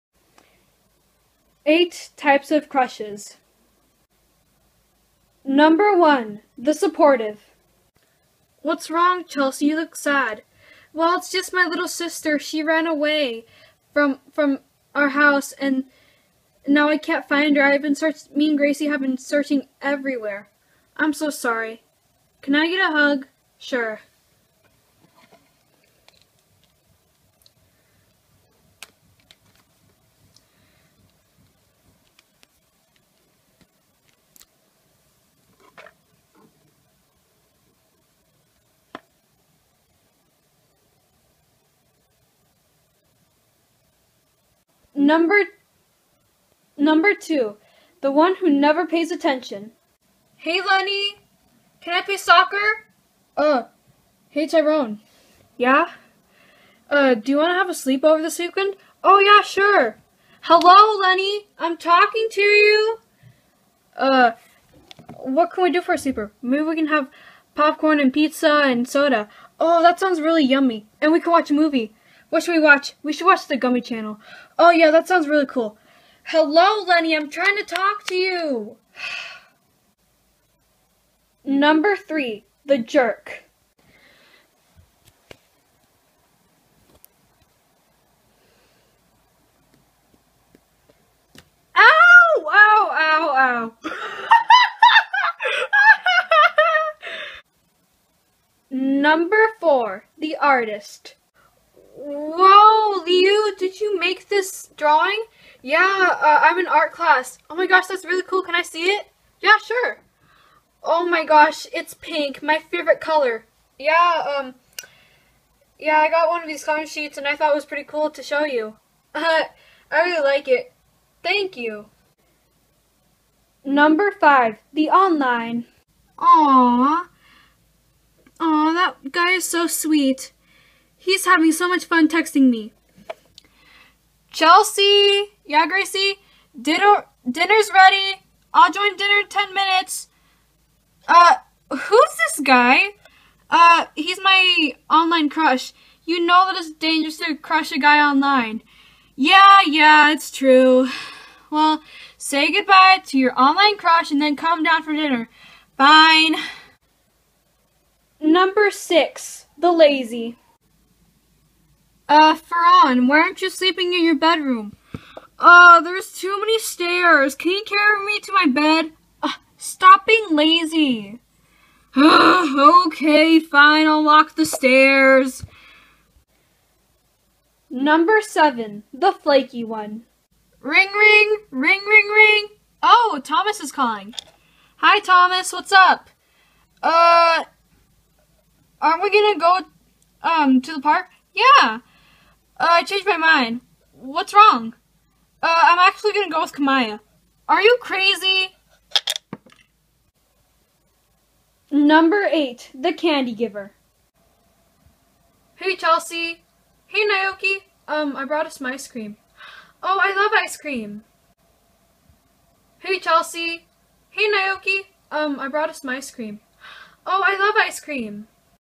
8 types of crushes
these are some types of crushes that i had and some that my friends had.